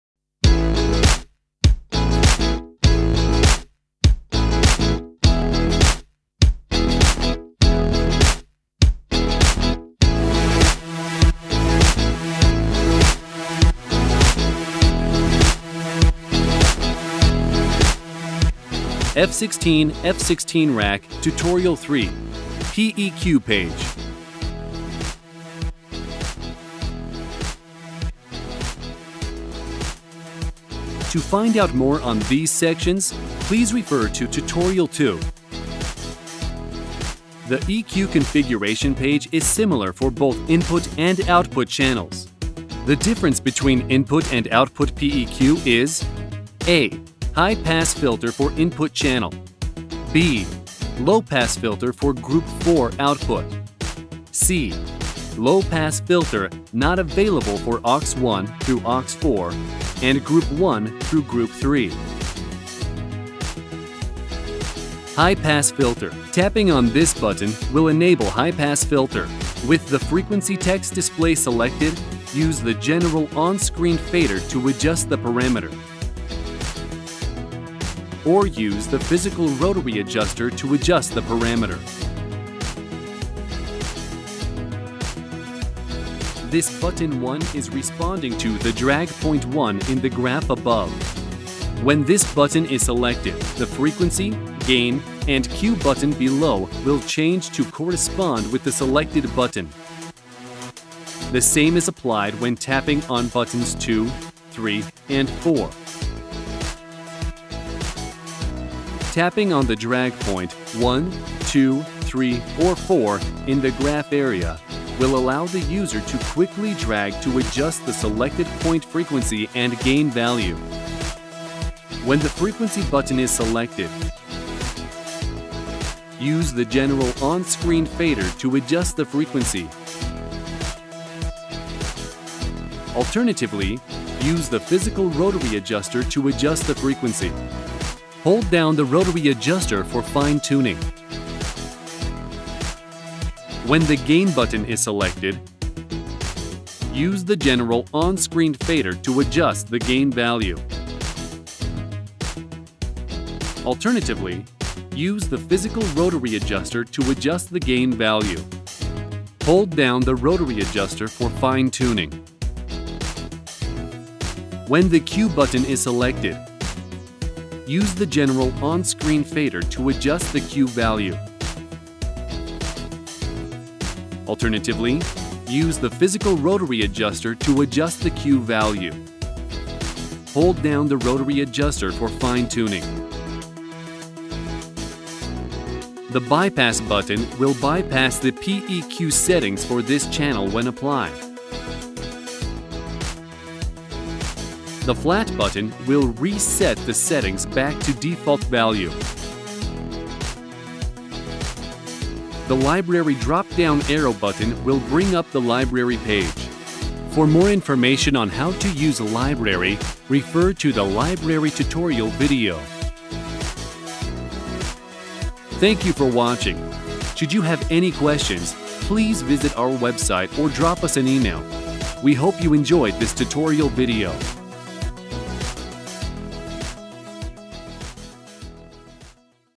F16 Tutorial 03 - EQ Page
In this tutorial, it introduces the EQ settings page. This applied to both F16 and F16rack model.